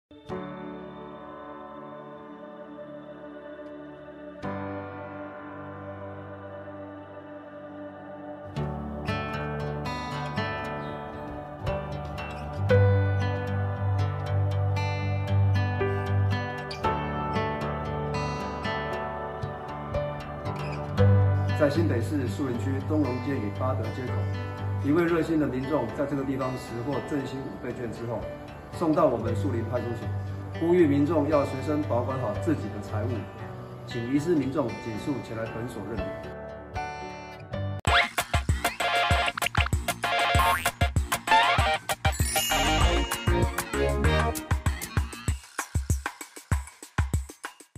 振興五倍券開領首日就「變不見」 民眾撿起關鍵1動作超暖心
記者陳豐德 翻攝/翻攝 我是樹林人